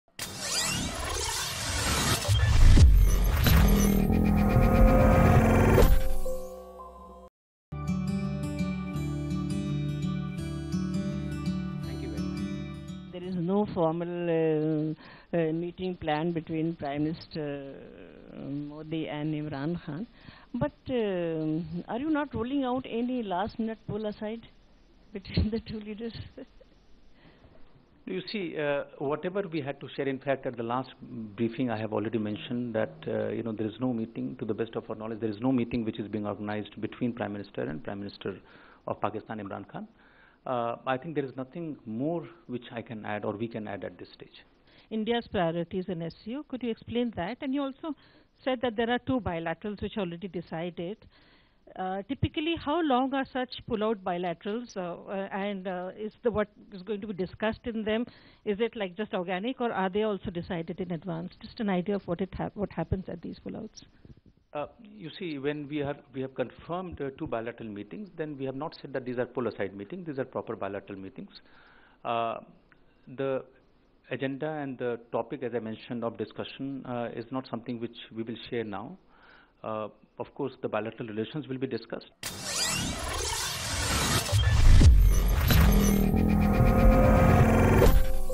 विदेश मंत्रालय ने PM Modi और Imran की मुलाकात पर दे डाला बड़ा बयान
विदेश मंत्रालय के प्रवक्ता रवीश कुमार ने कहा कि एससीओ समिट में पीएम मोदी और पाकिस्तान के प्रधानमंत्री इमरान खान के बीच कोई वार्ता नहीं होगी।